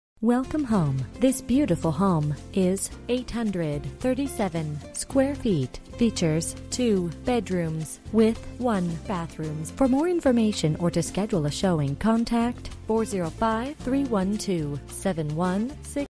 Residential for sale - 218 S Lahoma Avenue, Norman, OK 73069
Investor Special! AS-IS. Please submit all offers by May 27th. This home is being offered in conjunction to 216 S Lahoma. Can be sold separately or together. Per Seller, previously rented each home for $800-$900/month.

Bedrooms: 2
Bathrooms: 1
Square Feet: 837
Price: $89,500